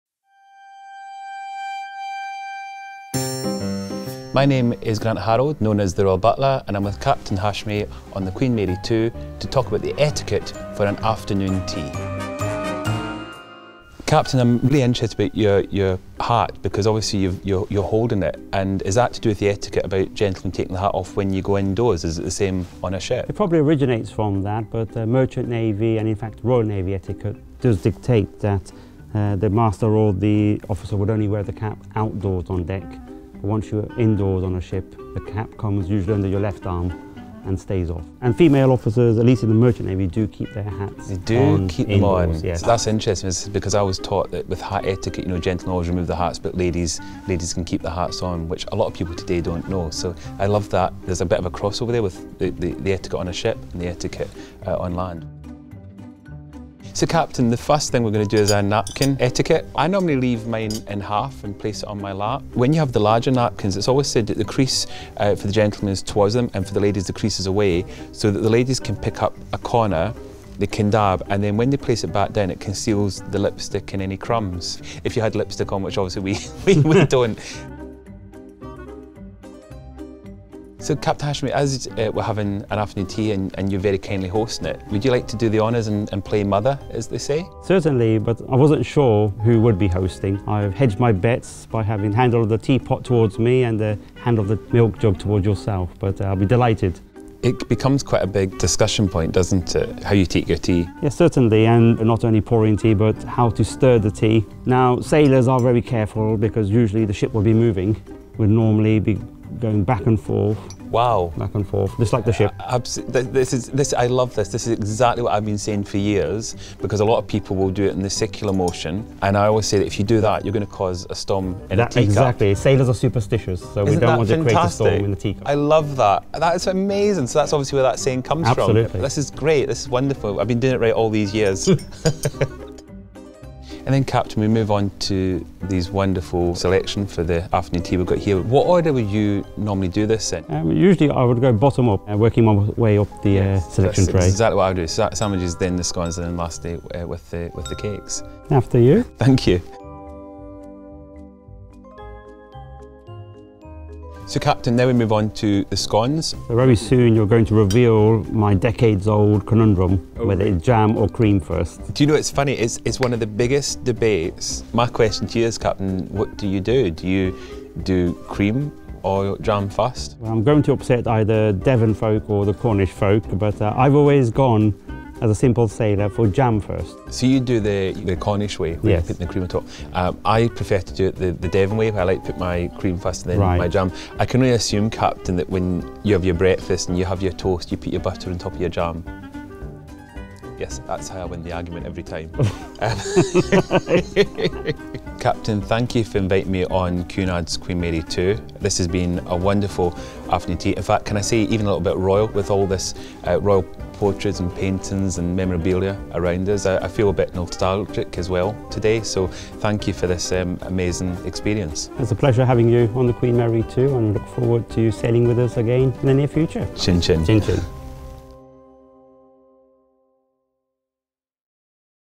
Cunard | Afternoon Tea with Grant Harrold & Captain Aseem Hashmi
This week we are excited to be celebrating World Afternoon Tea Week!

And to make sure we are celebrating in the right way, Captain Aseem Hashmi teamed up with the UK’s pre-eminent etiquette expert Grant Harrold.

So who’s side are you on? Team Clotted Cream first or Team Jam First?